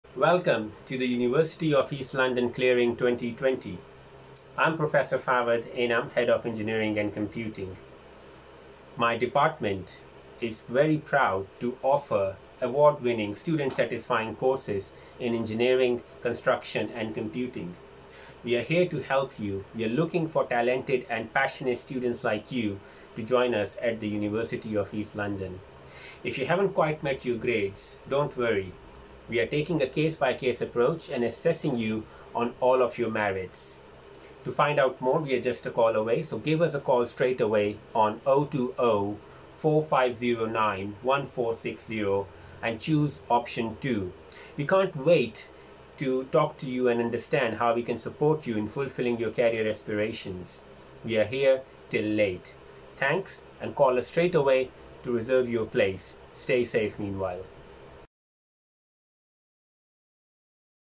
Prof. Fawad Inam, Head of Engineering and Computing guides students on how to apply for 2020 entry.
At the University of East London we take a holistic approach to applications and will look at other achievements as well as academic grades. "We can’t wait to talk to you and understand how we can support you in fulfilling your career aspirations.